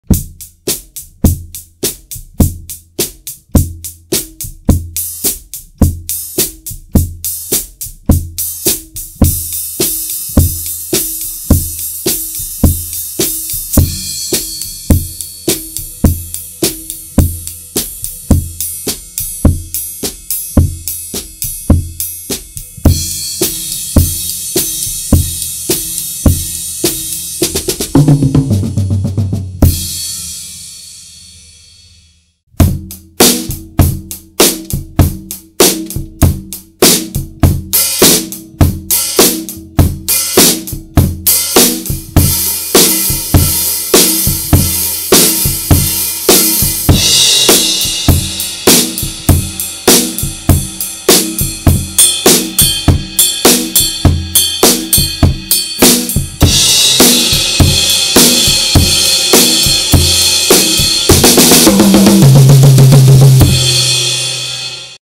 Practice Set Up vs Regular Set Up (NO TALKING) - Liam Fitz On Drums
Equipment used:
Ludwig drums
Zultan cymbals
EVANS drum heads
Collision drumsticks